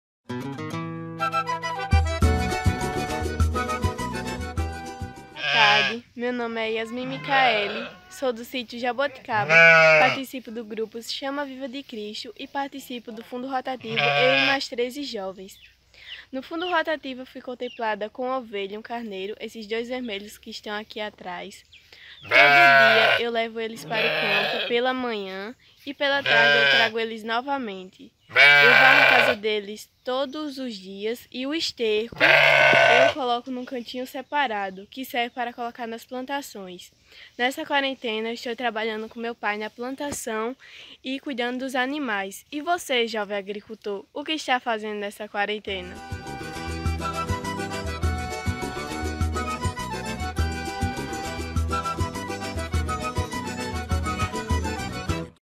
Yasmim Mickaelly
Estamos vivendo uma pandemia. A juventude do Polo da Borborema está em casa, mas não está parada. Saiba o que Yasmim está fazendo durante seu isolamento social.